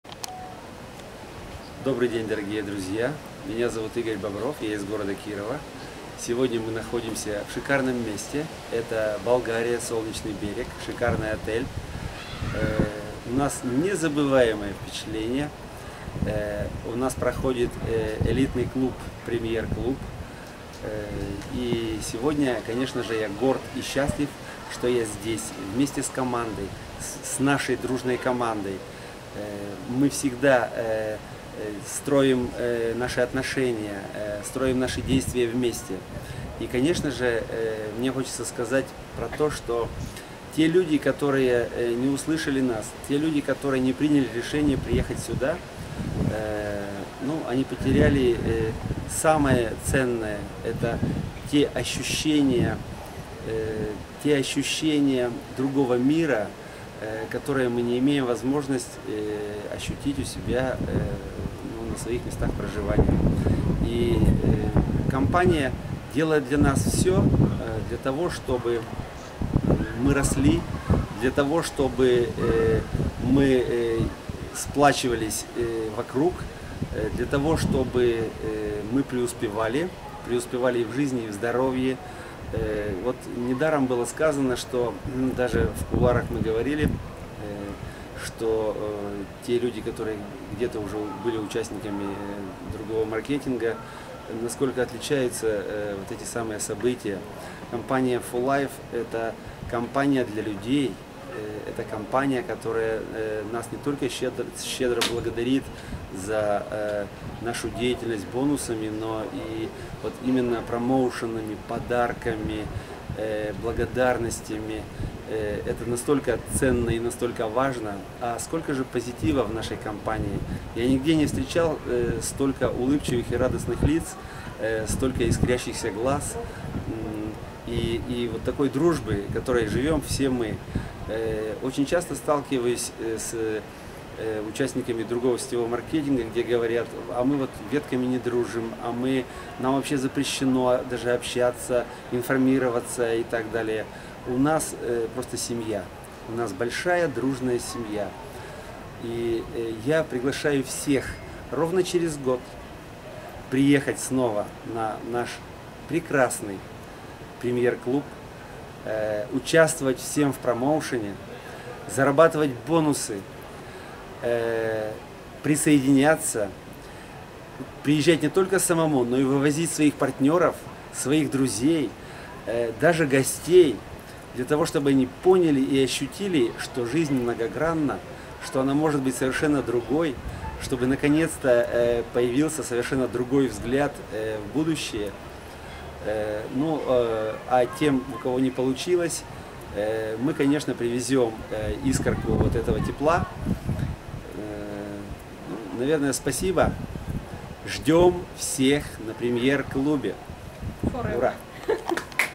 О Премьер Клубе компании 4Life, Игорь Бобров, Президент Даймонд
С 5 по 7 августа 2016 г. компания 4Life пригласила своих дистрибьюторов рынка Евразии на элитное мероприятие "Премьер Клуб" , которое состоялось на Солнечном берегу в Болгарии.  Это были незабываемые дни!